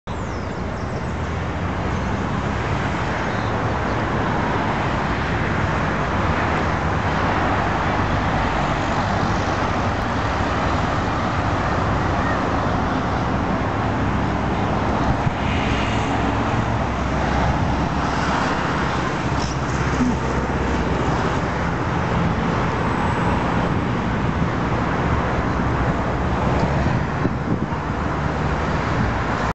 Petit Chaperon rouge
Monument au Petit Chaperon rouge, à Palermo, l'un des plus beaux quartiers de Buenos Aires, en Argentine.